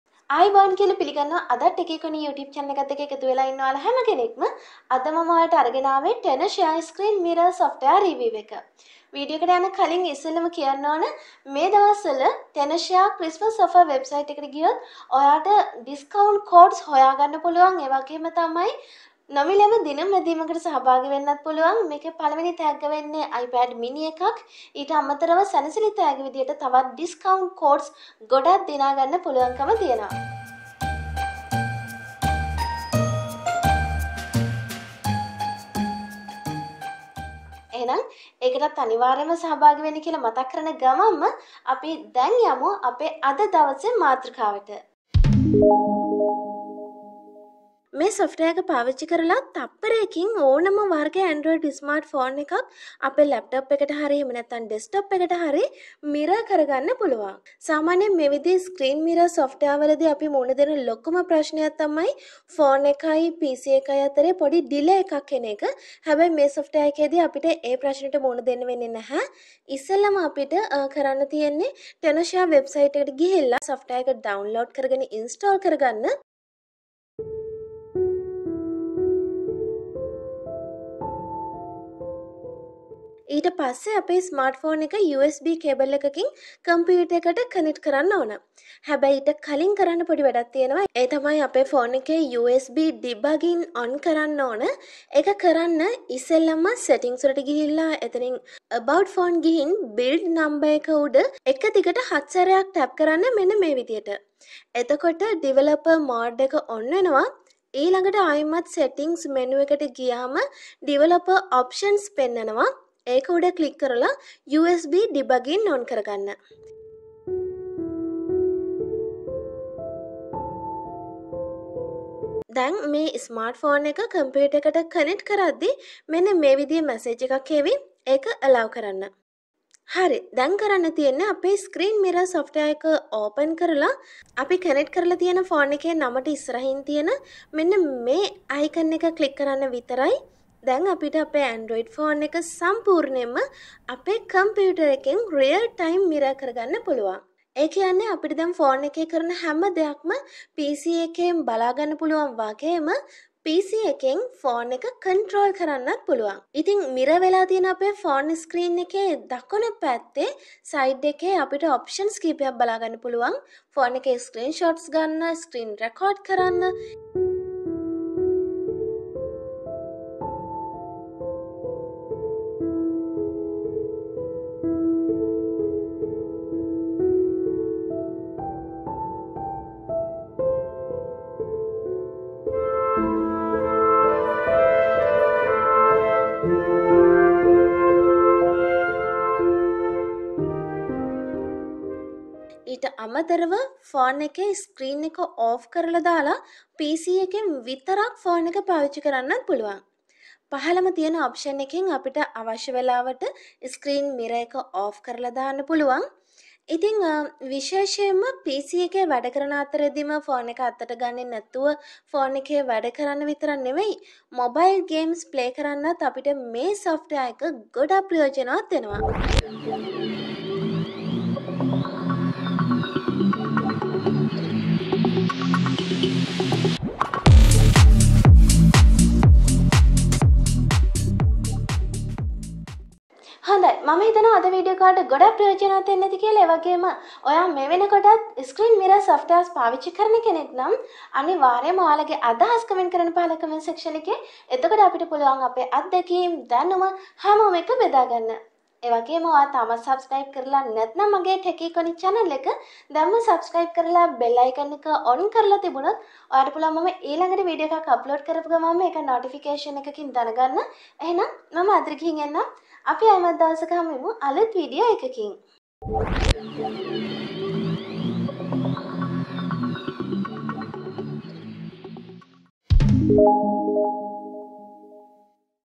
Top-Rated Software for Screen Mirroring Android to PC [No Delay] | Tenorshare Phone Mirror
Want to play mobile games on PC? How to mirror Android screen and display it on a computer?
Feel free to use Tenorshare Phone Mirror. Tenorshare Phone Mirror is one of the top-rated screen mirroring software for Android to PC.
With Phone Mirror, you can mirror Android to PC in real-time & no delay.